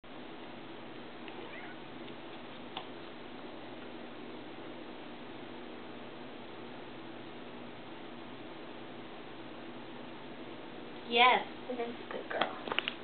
Ed the cat learning to Wave Hello
Here is a little of the process in training her to wave. I normally use a clicker but we were trying to film the final behavior so I did a quick refresher session and we got that on film.